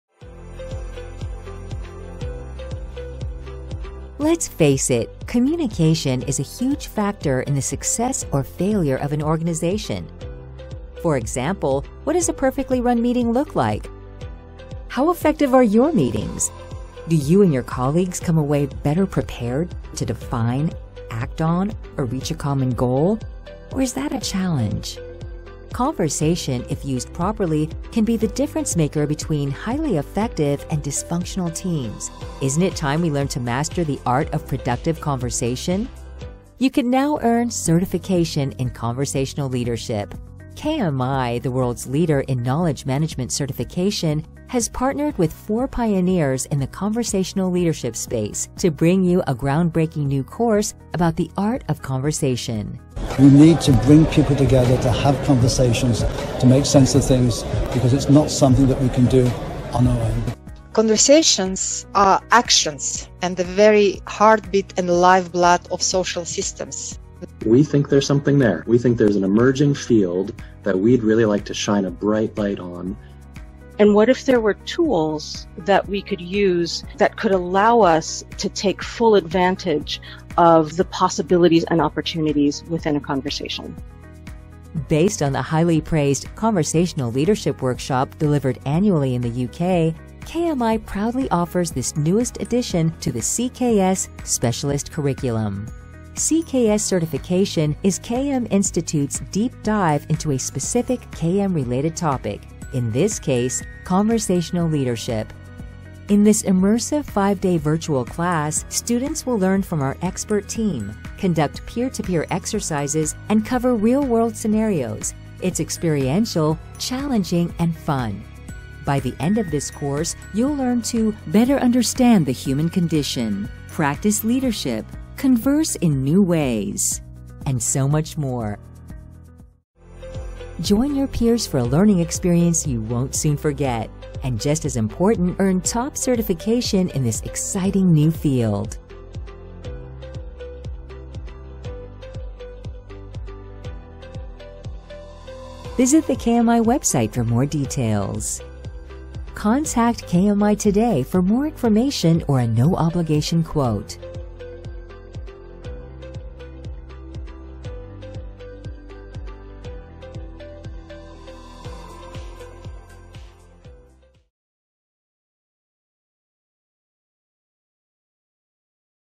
Earn CKS Certification in Conversational Leadership from KMI (Next class - TBD in Early 2025!)
New! Certified Knowledge Specialist in Conversational Leadership
Next Virtual Class: TBD in early 2025!

Now you can earn Certification in Conversational Leadership - the fast-growing and evolving art of conversation to improve communication in the workplace as well as our everyday lives.  Based on the highly praised "Conversational Leadership Workshop" delivered annually in recent years; KMI is proud to offer the newest addition to the CKS (Specialist) curriculum.

00:00 - Communications, Perfect Meetings
00:14 - How effective are yours?
00:36 - Welcome to KMI's CKS-CL Certification!
00:40 - Meet your Facilitators
00:52 - David Gurteen
01:00 - Saule Menane
01:06 - John Hovell
01:13 - Donita Volkwijn
01:45 - Course Highlights
01:58 - What you will learn
02:10 - Register for our next class today!